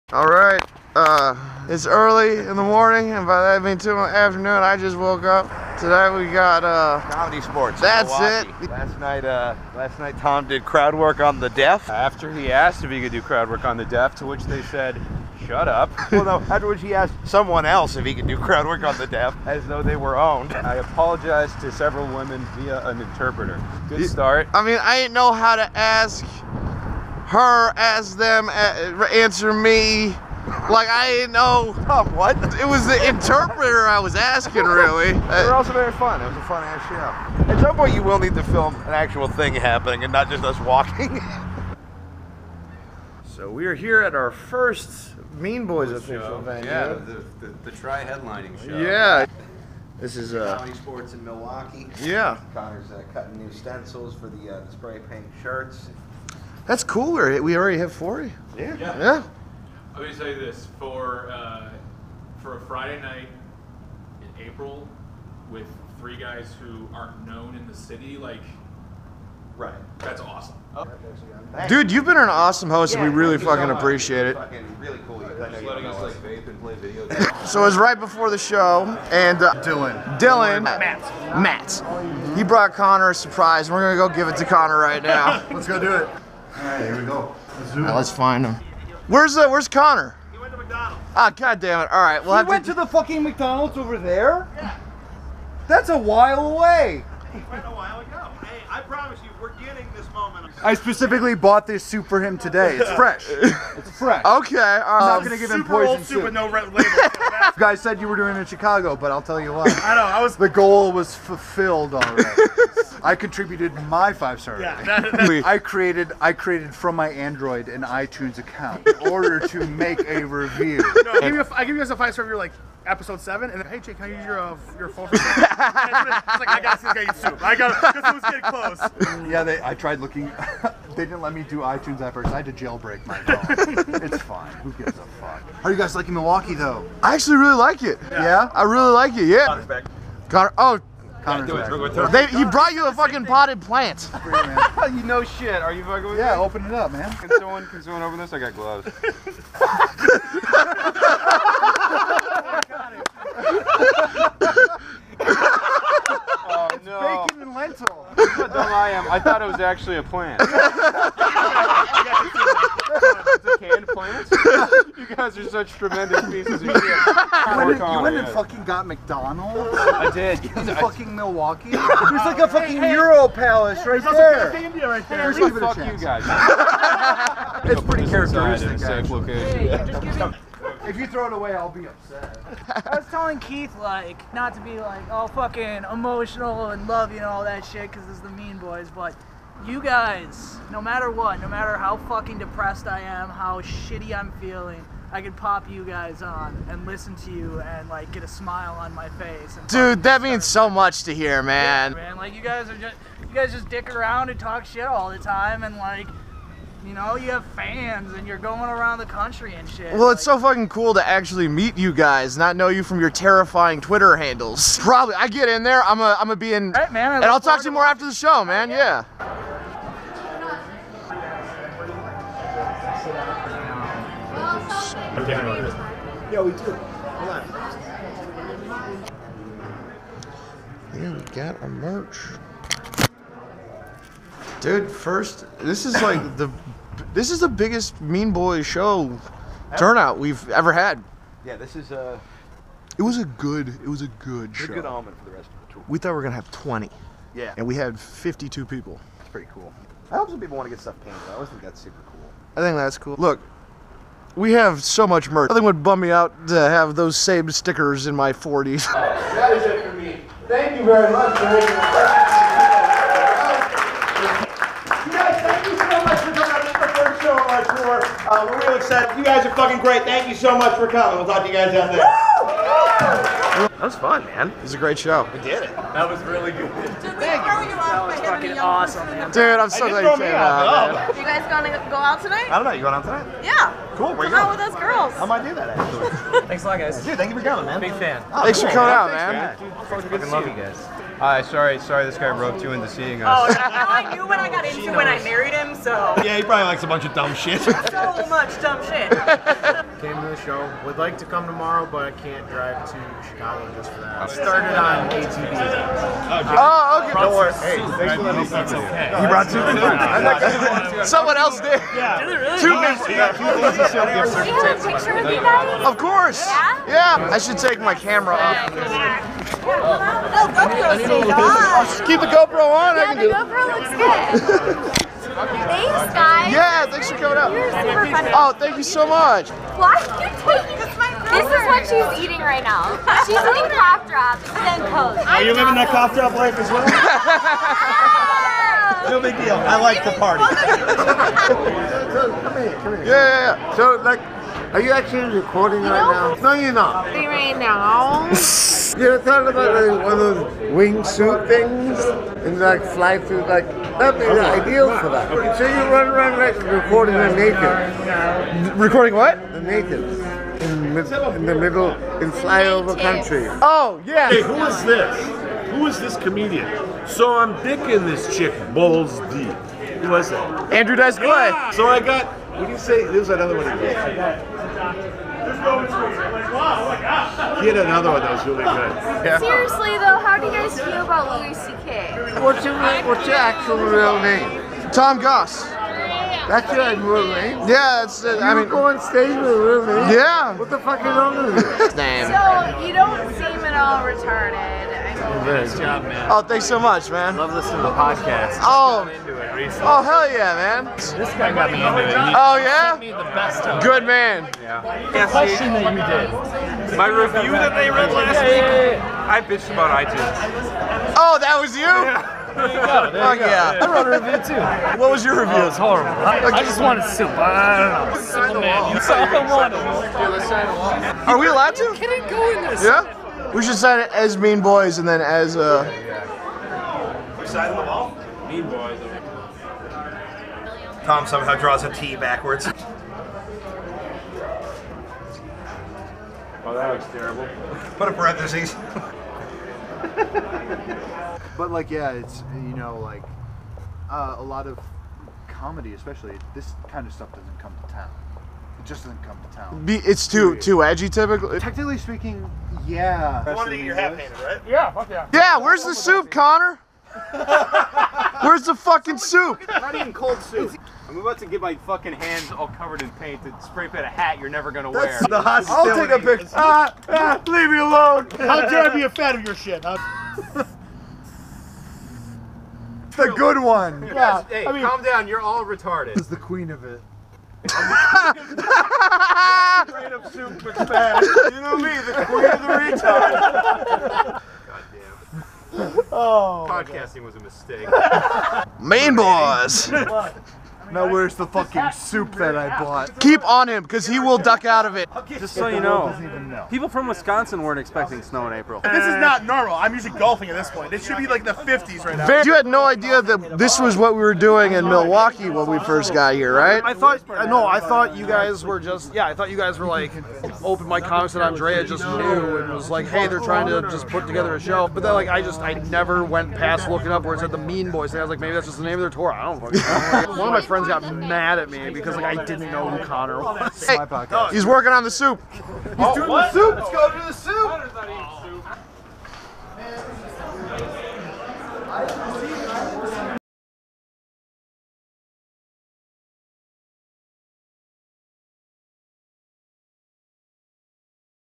VLOG #2: Soup War in Milwaukee
Day 3 of the tour. From the show at the Comedy Sportz in Milwaukee Wisconsin.